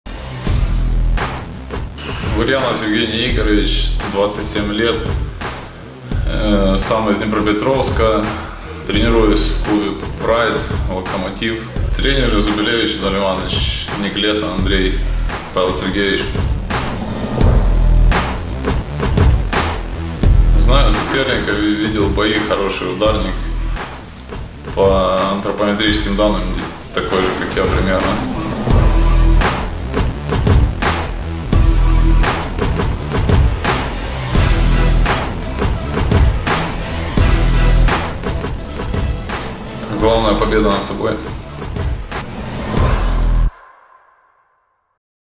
Мастер спорта по ММА Евгений Гурьянов
👊️Участник Международного турнира по фри-файту "Битва за честь"
🇺🇦️ Украина, г. Днепр